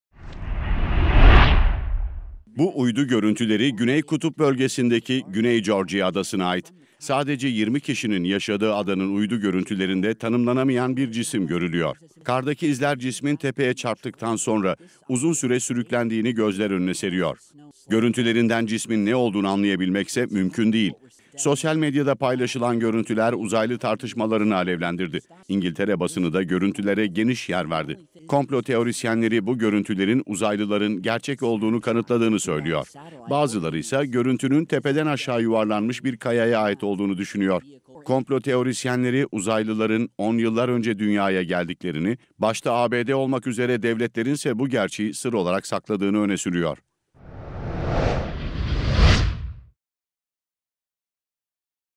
Gizemli cisim tartışma yarattı (Yere çakılmış UFO olduğu iddia ediliyor)
Sosyal medyada paylaşılan bir uydu görüntüsü uzaylılar konusundaki tartışmaları yeniden alevlendirdi. Güney Kutbu yakınlarındaki küçük bir adada görüntülenen cismin yere çakılmış bir uzay gemisi olduğu iddia ediliyor.